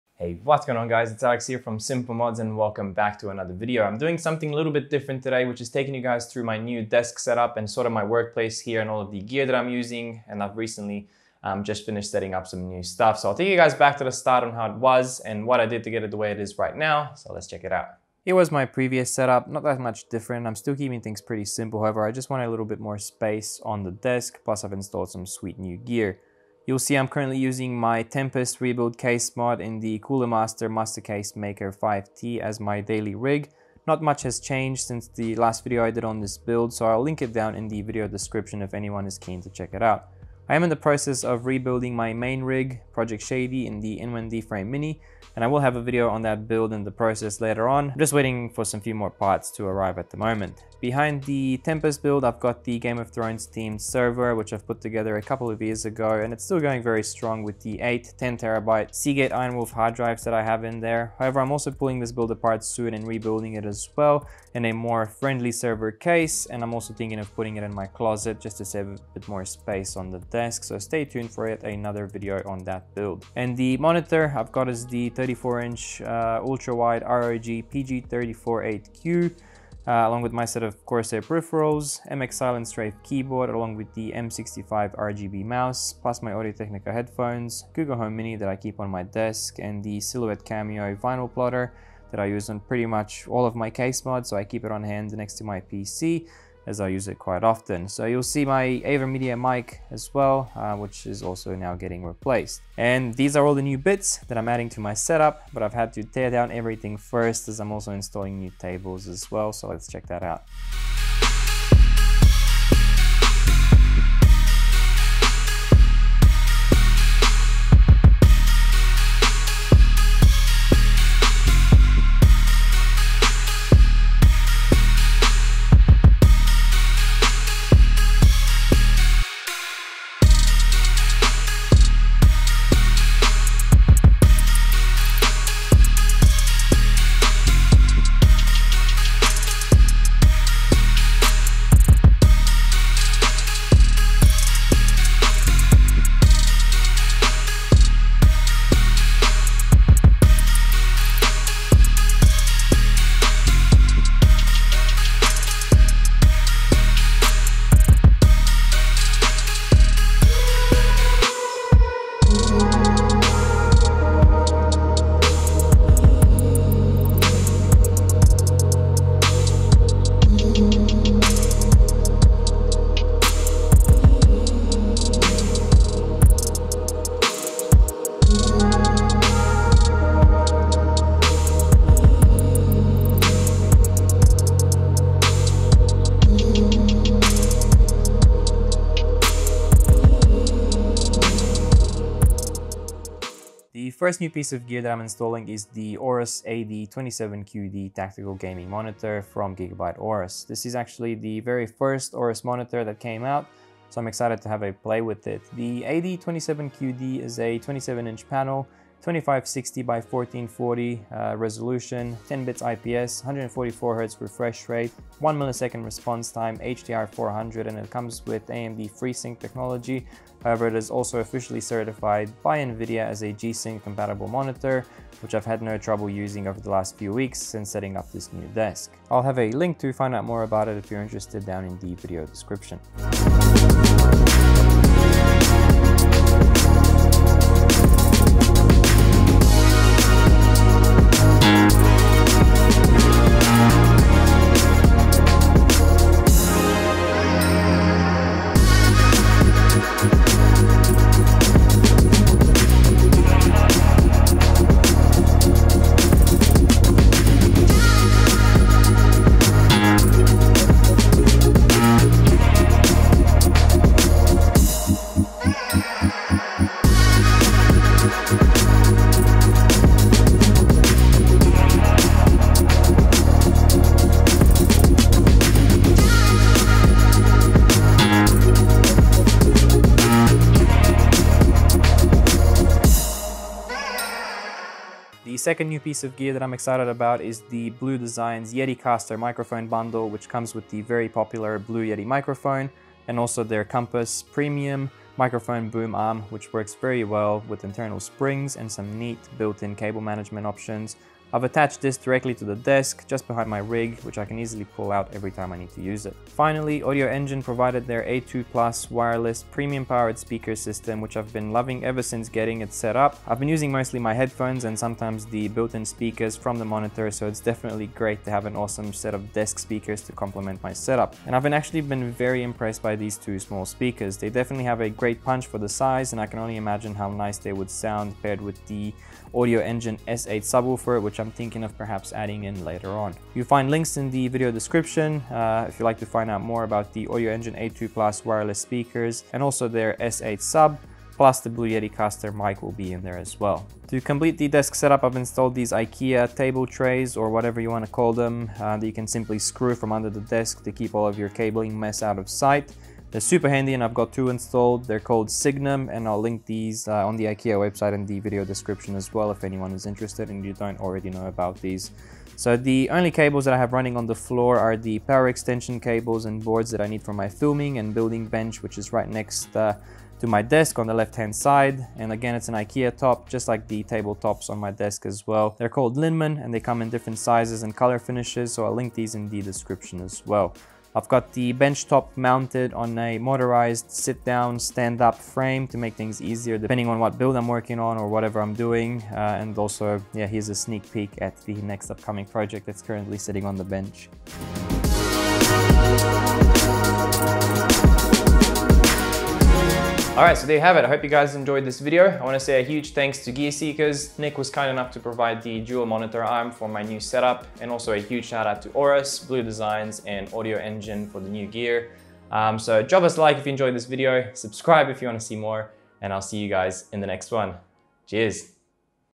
New SimpleModz Desk Set-Up
Hey guys! Here's my new desk set-up and workplace for creating my case mods! Hope you enjoy this different kind of video!

Ikea Signum

Ikea Linmon